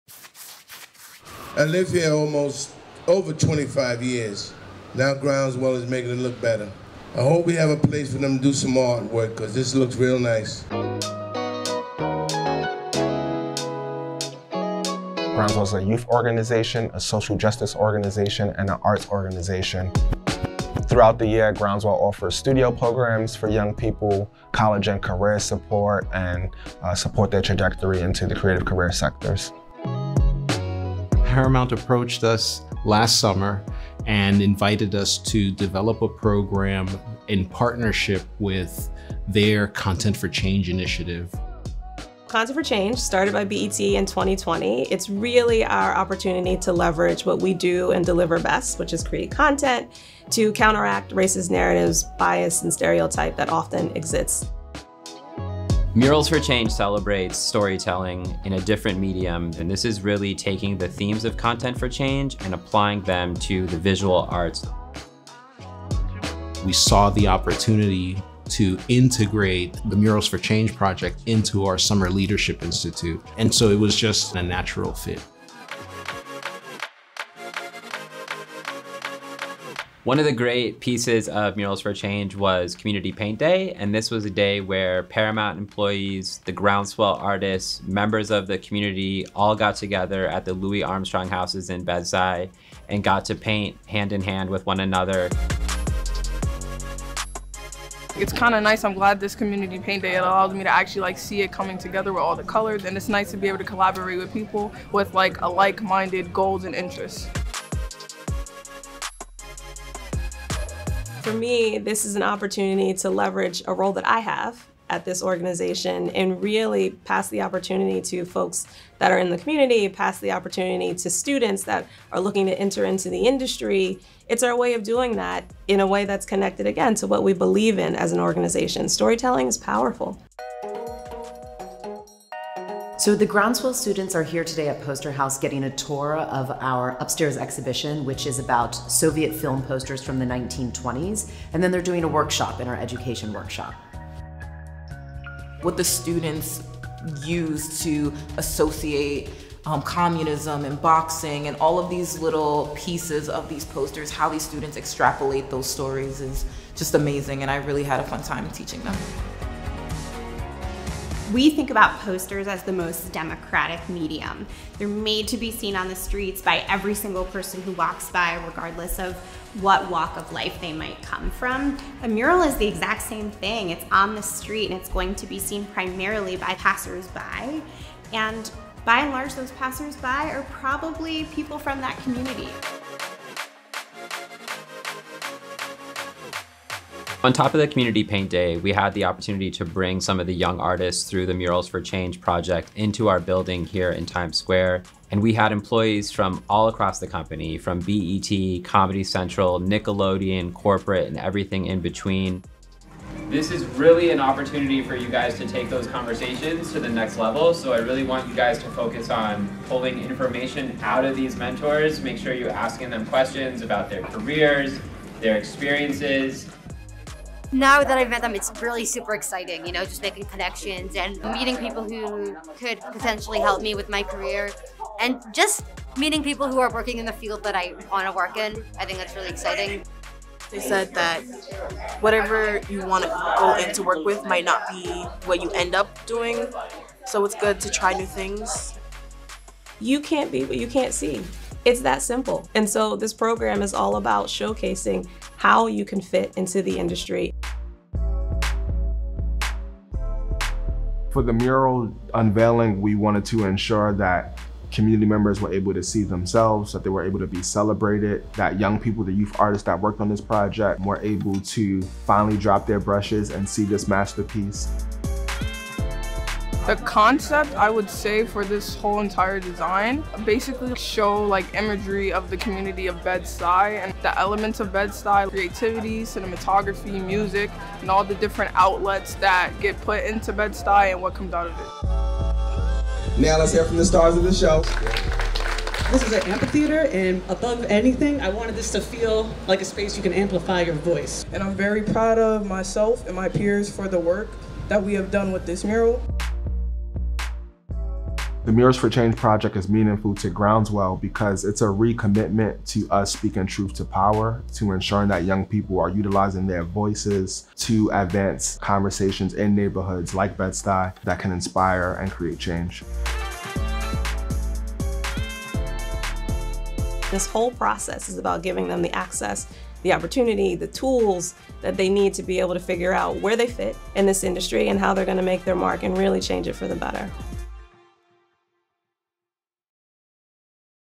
Groundswell | Murals for Change
Groundswell is the inaugural partner of Content for Change, Murals for Change initiative that brings together artists, youth, and community organizations to use art & murals as a tool for social change.
Through Murals for Change, Paramount employees and young teaching artists from NYC came together to unlock imaginations and empower the community of Bedford-Stuyvesant with a new mural finalized in September of 2022. ​